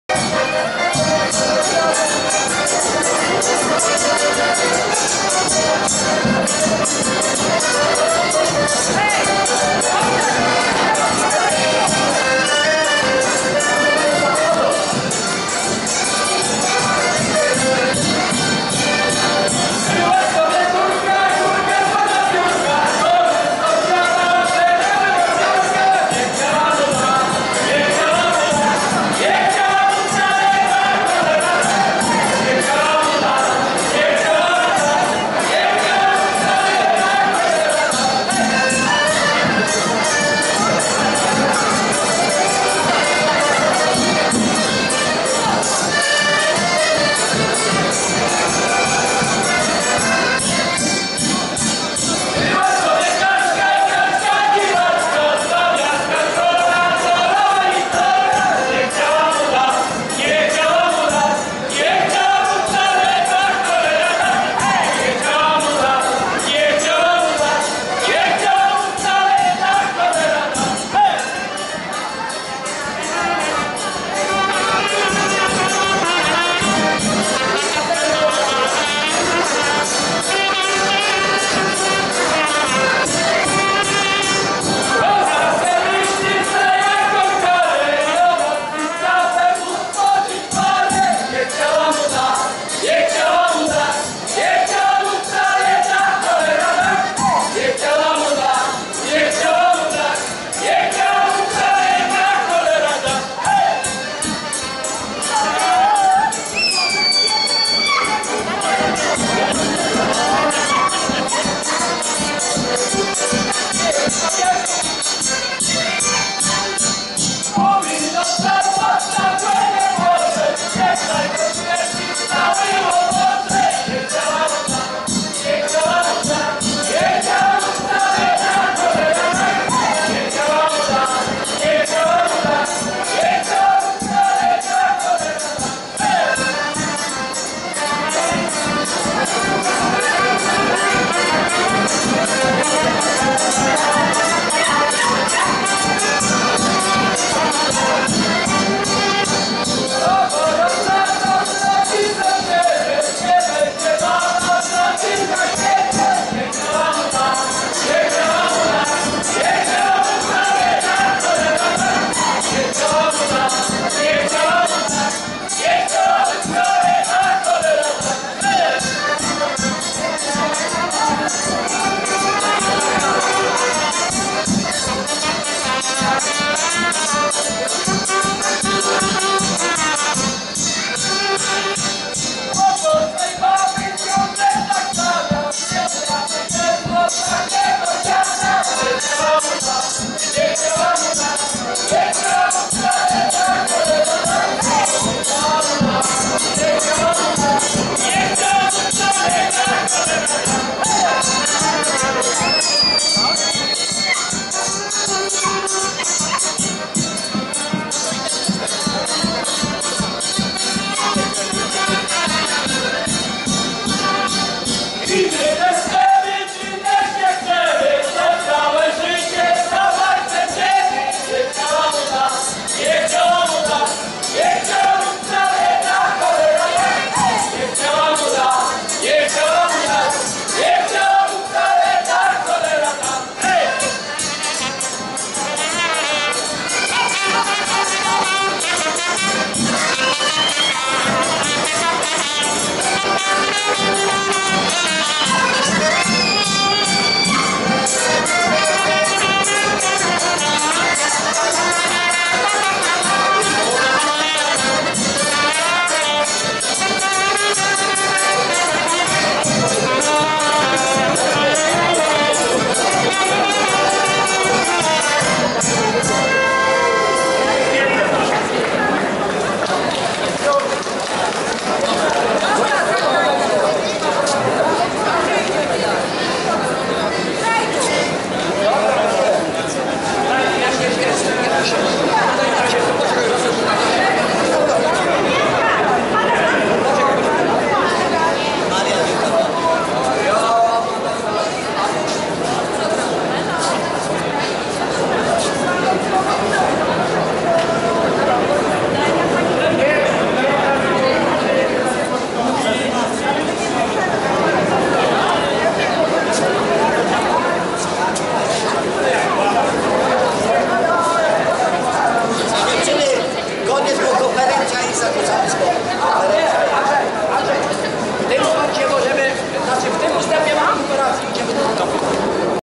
Kapela na stoisku Doliny Strugu - ECOFAMILY 2016
V Targi Ekologia dla Rodziny ECOFAMILY - Kielce 2016
Gra kapela Wierna Rzeka